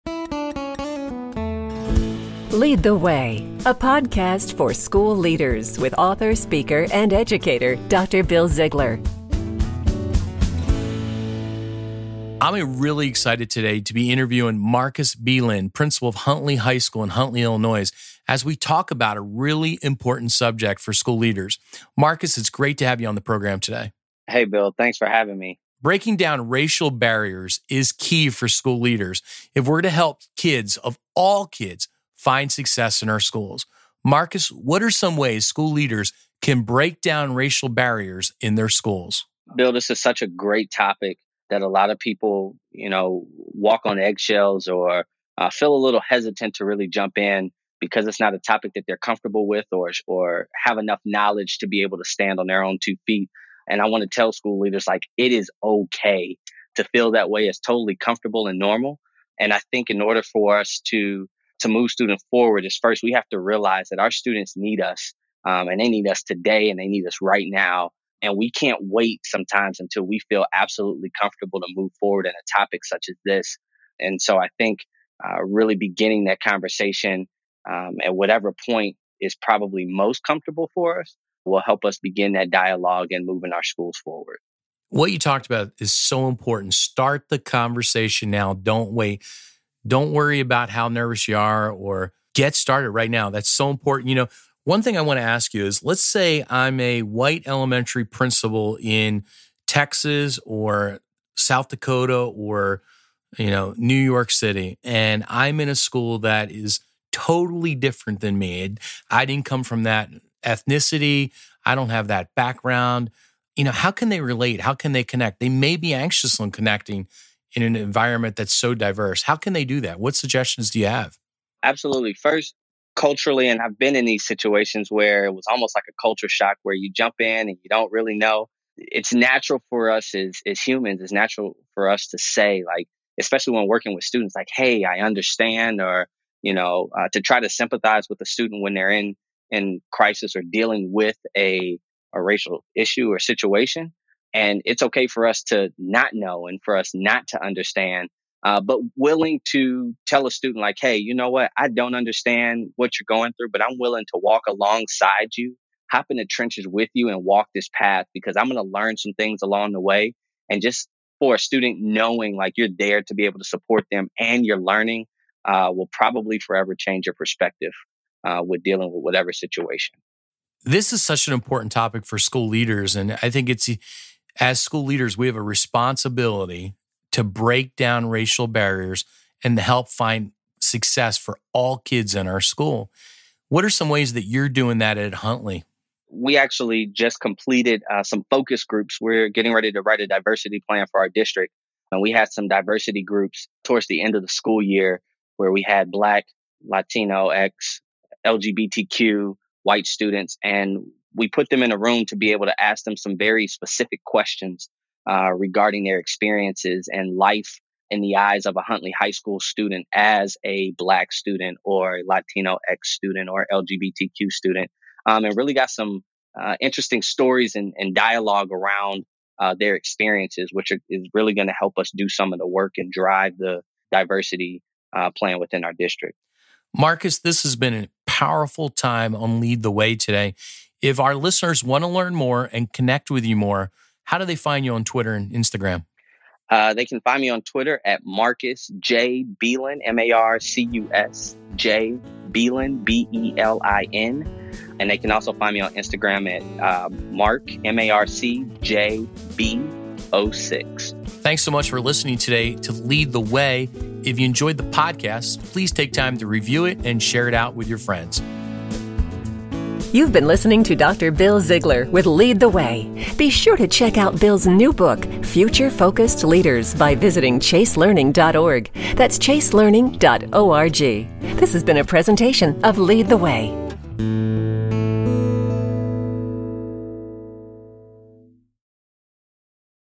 #1 Breaking Down Racial Barriers with Marcus Belin, Principal of Huntley High School
#1  Marcus Belin shares how school leaders need to break down racial barriers to help all students find success.